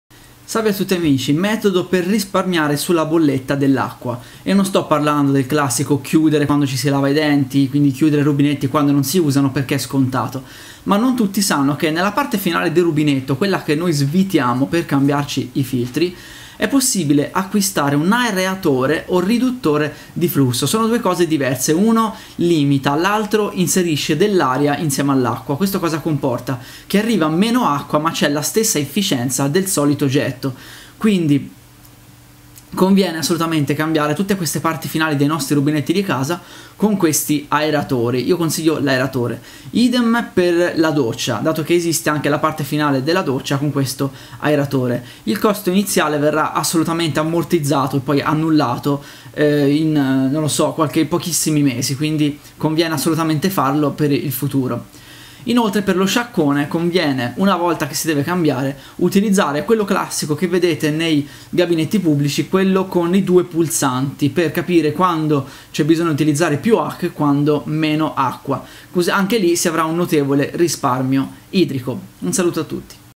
Come risparmiare sulla bolletta dell'acqua
Vediamo cosa possiamo fare per ridurre il consumo idrico.